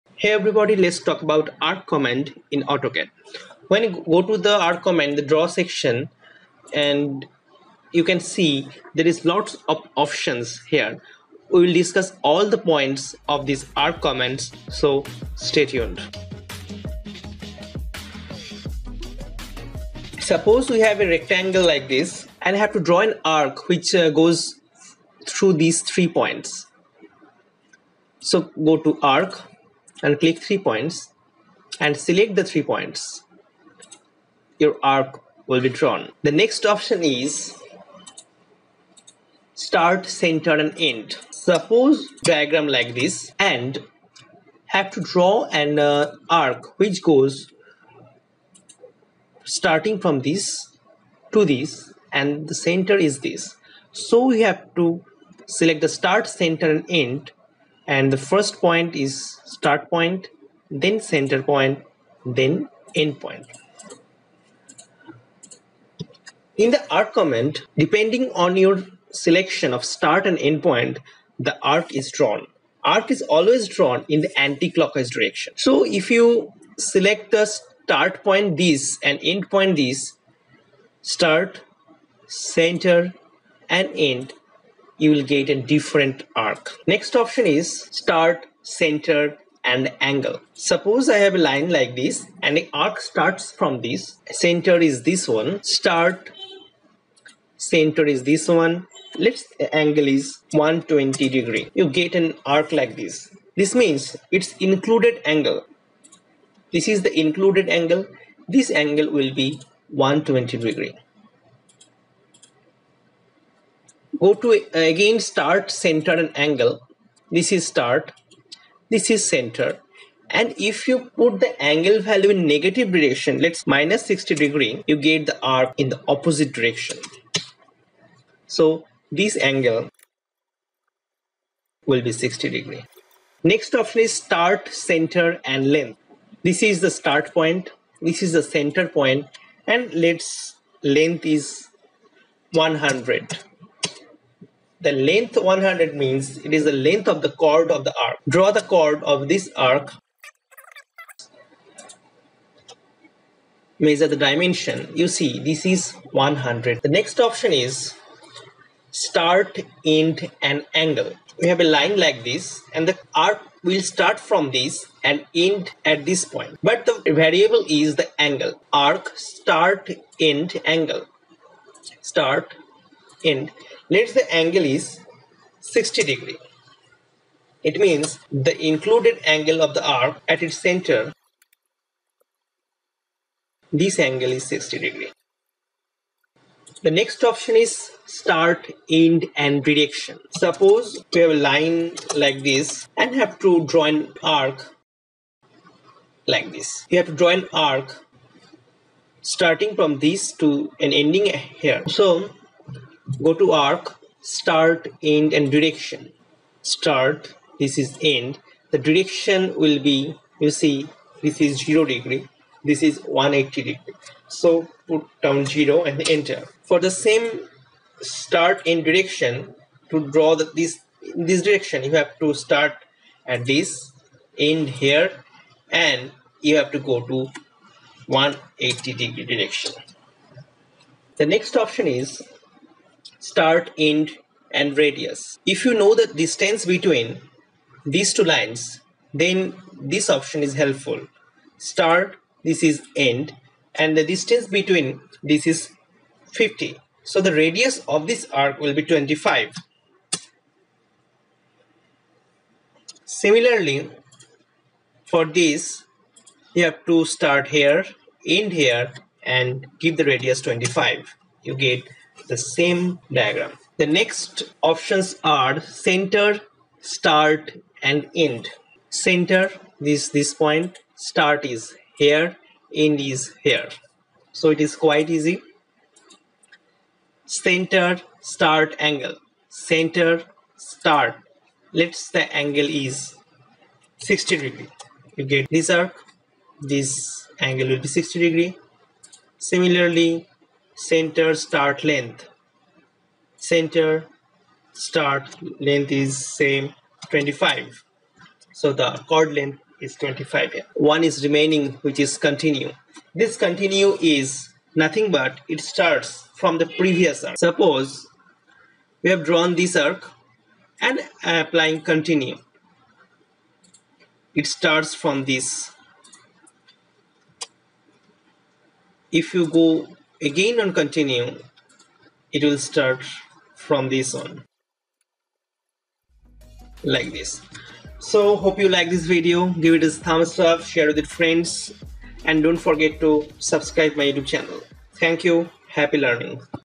How to Draw Arc in AutoCAD (All types explained)
This video explains all procedure of drawing arc in AutCAD.